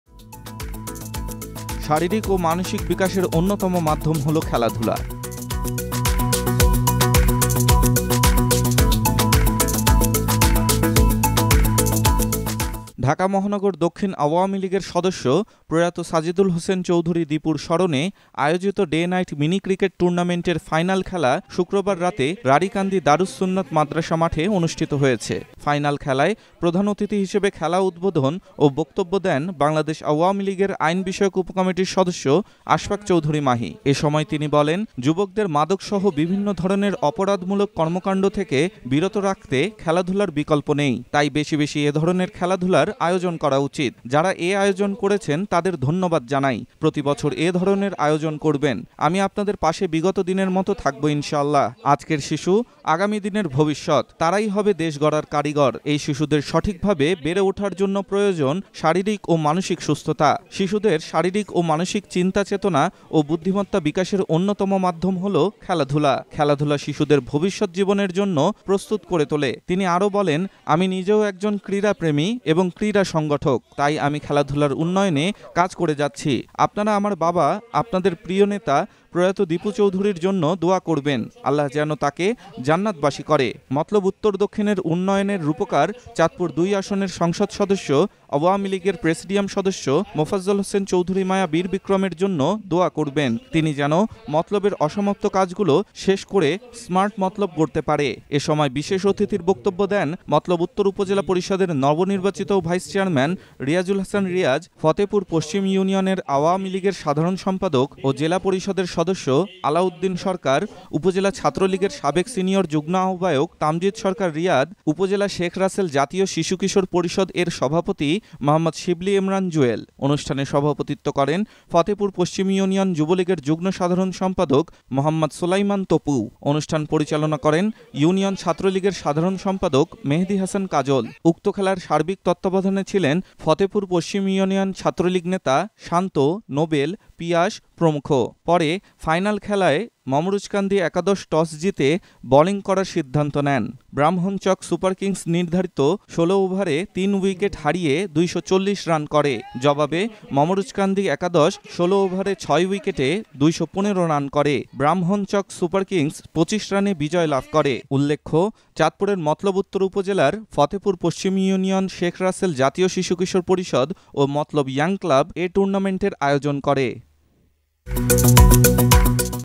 ‘শারীরিক ও মানসিক বিকাশের অন্যতম মাধ্যম হলো খেলাধুলা’
ঢাকা মহানগর দক্ষিণ আওয়ামী লীগের সদস্য প্রয়াত সাজেদুল হোসেন চৌধুরী দিপুর স্মরণে আয়োজিত ডে-নাইট মিনি ক্রিকেট টুর্নামেন্টের ফাইনাল খেলা শুক্রবার (৩১ মে) রাতে রাঢ়ীকান্দি দারুস সুন্নত মাদ্রাসা মাঠে অনুষ্ঠিত হয়েছে।